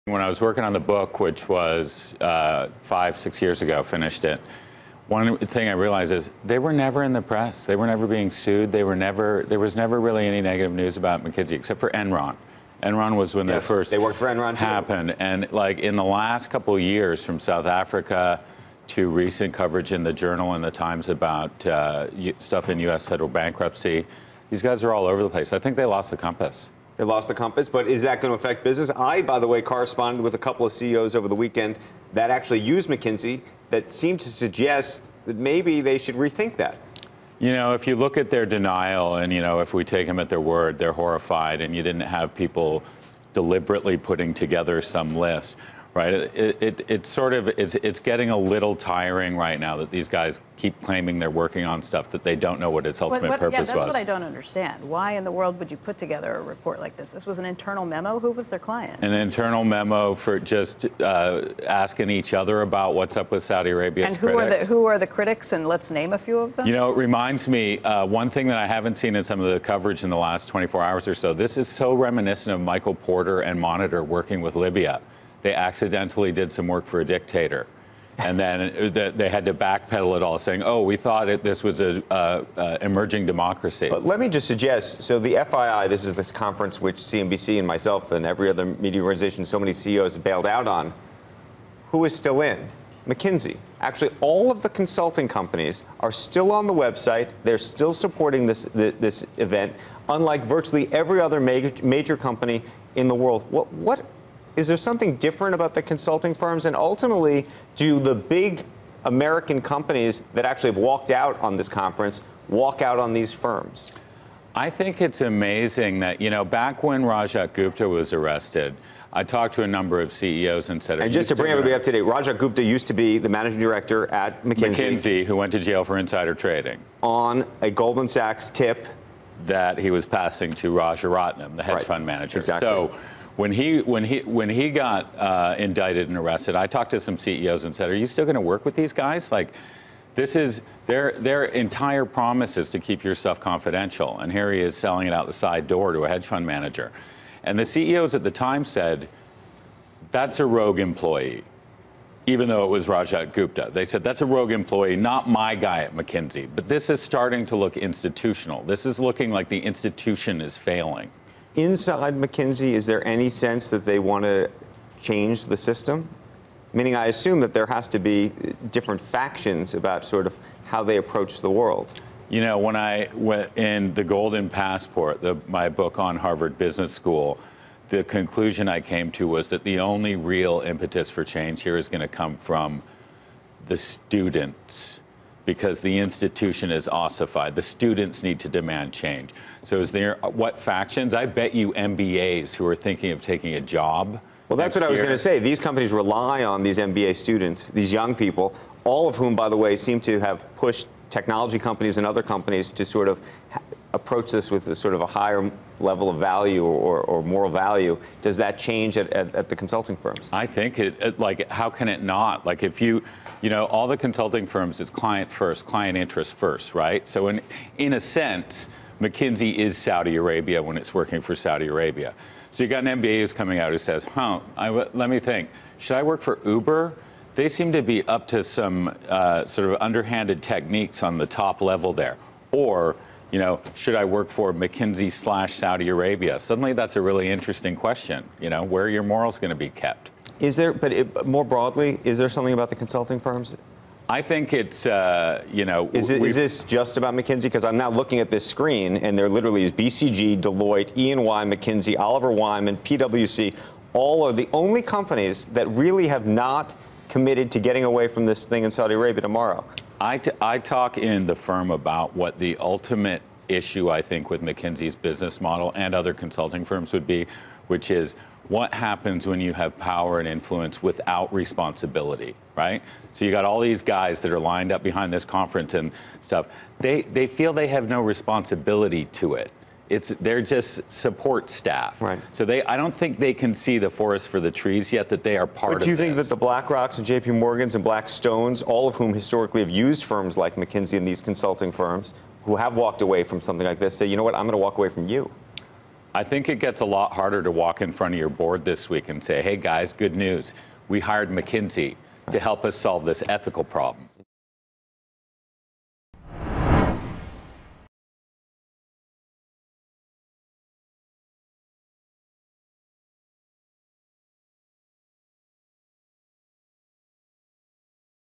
This is starting to look institutional, says expert on McKinsey Saudi report
Duff McDonald, “The Firm” and "The Golden Passport" author, discusses consulting firm McKinsey & Co. and news out of "The New York Times" about a McKinsey report on public perception of Saudi policies potentially used to silence dissidents.